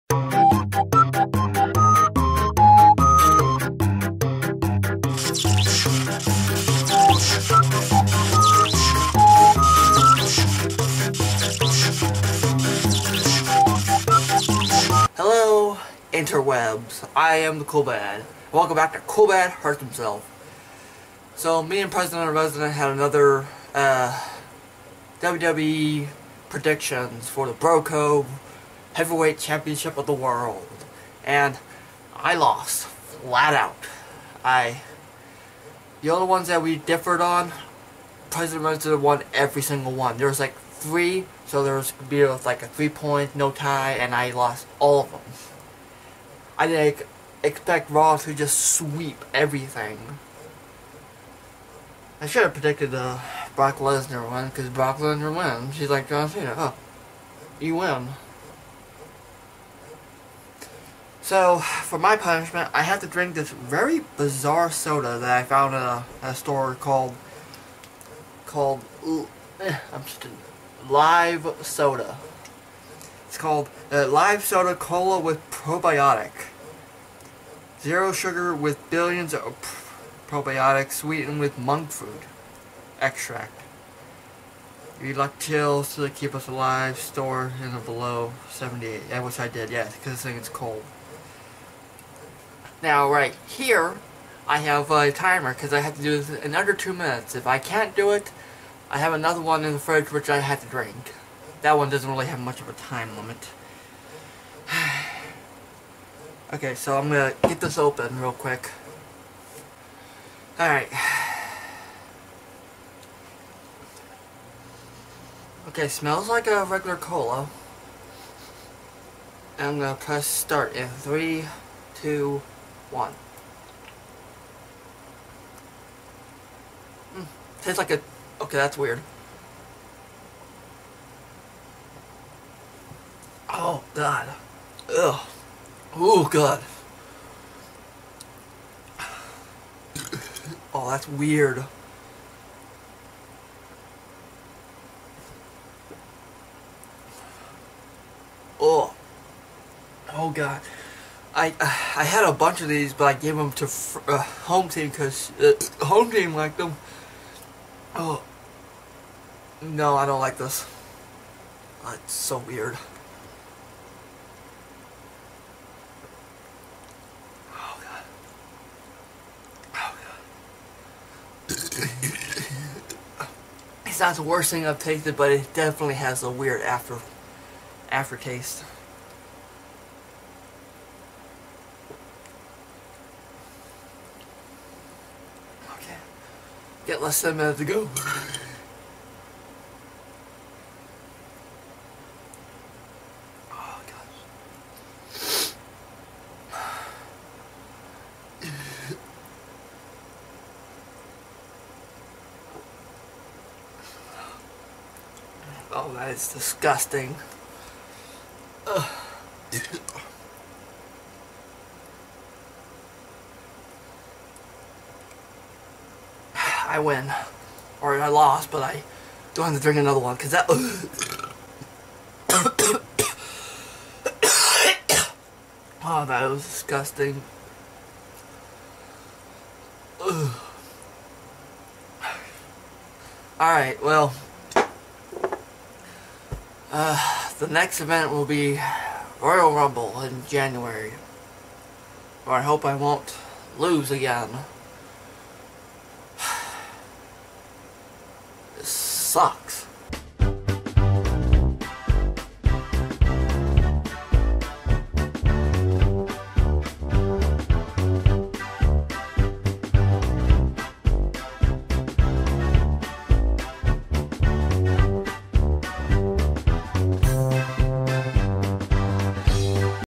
CoolBad Hurts Himself Episode 22: Live Soda
CoolBad loses another bet and has to drink Live Soda

YouTube.Com Privacy